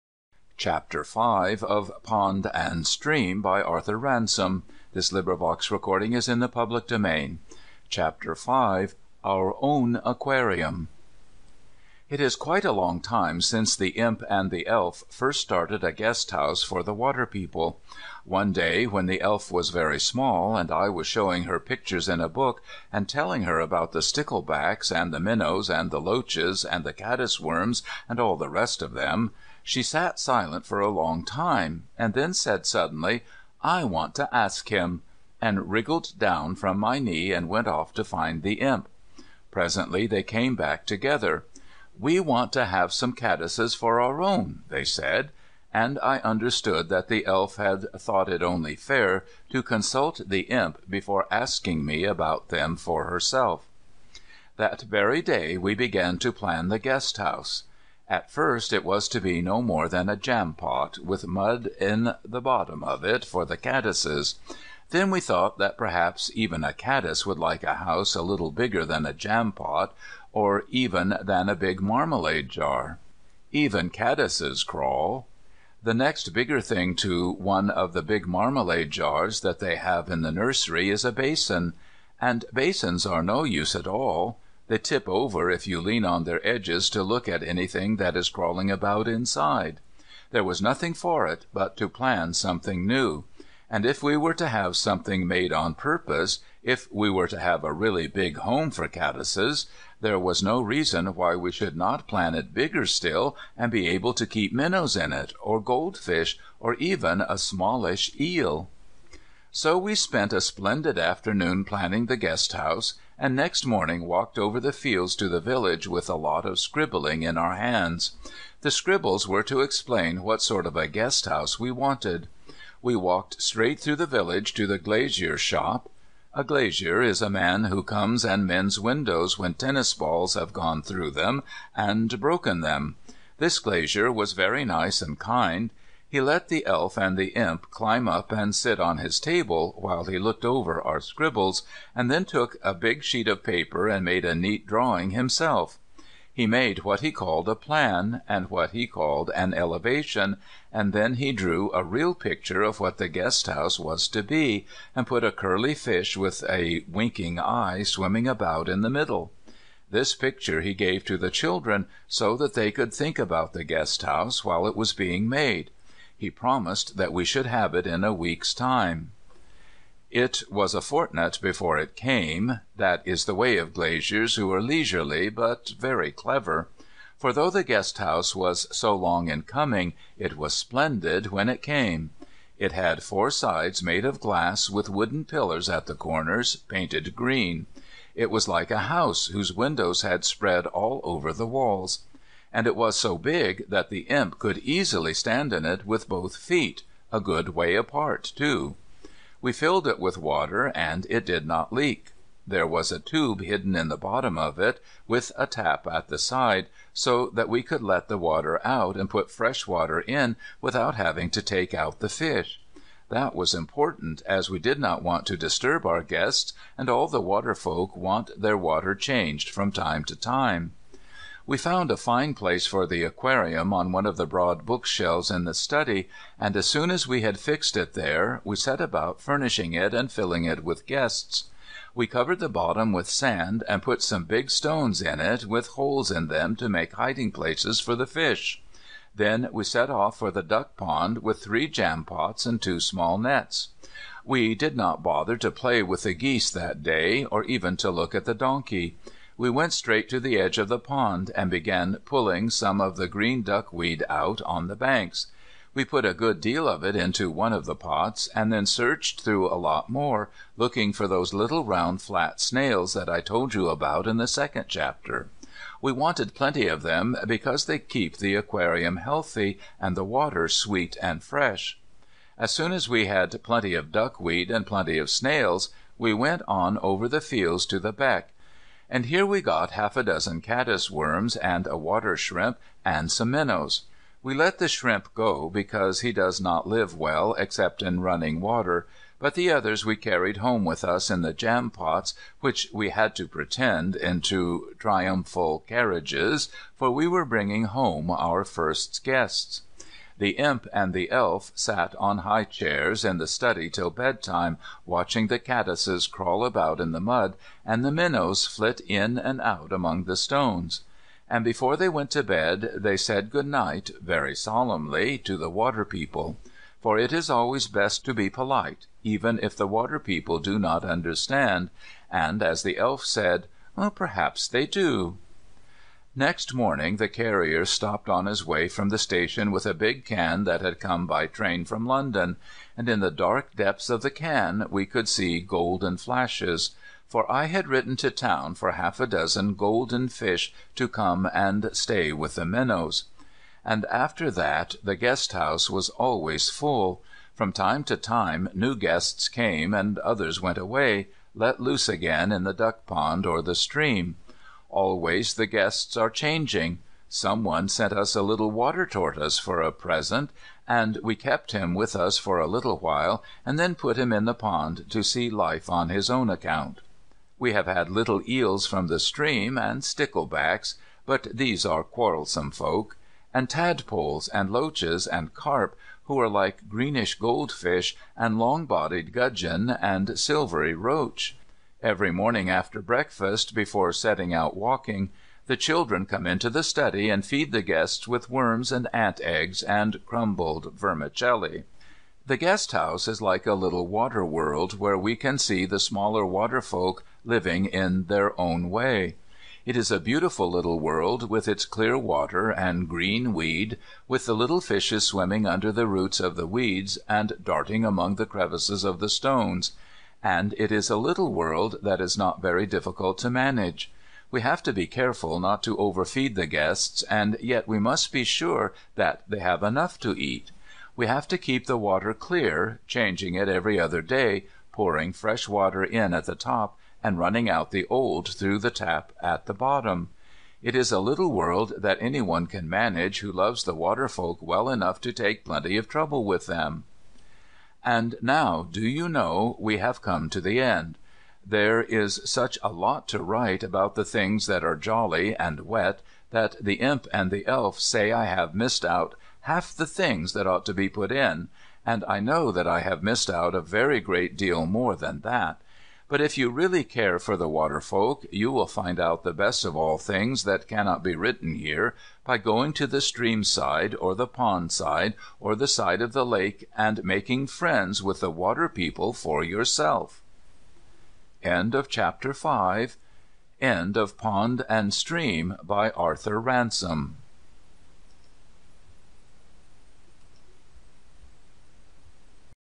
Arthur Ransome - Pond And Stream (5/5) Our Own Aquarium
POND AND STREAM.
This book is part of the author’s "Nature Books For Children" series (three books). The book is fun and charming, even for adults. This is a book about the things that are jolly and wet: streams, and ponds, and ditches, and all the things that swim and wriggle in them.

AUTHOR BIOGRAPHY.
Arthur Michael Ransome (January 18, 1884 – June 3, 1967) was an English author and journalist. He is best known for writing the Swallows and Amazons series of children's books about the school-holiday adventures of children, mostly in the Lake District and the Norfolk Broads. The books remain popular and "Swallows and Amazons" is the basis for a tourist industry around Windermere and Coniston Water, the two lakes Ransome adapted as his fictional North Country lake. He also wrote about the literary life of London, and about Russia before, during, and after the revolutions of 1917. His connection with the leaders of the Revolution led to him providing information to the Secret Intelligence Service while he was also suspected of being a Soviet spy by MI5.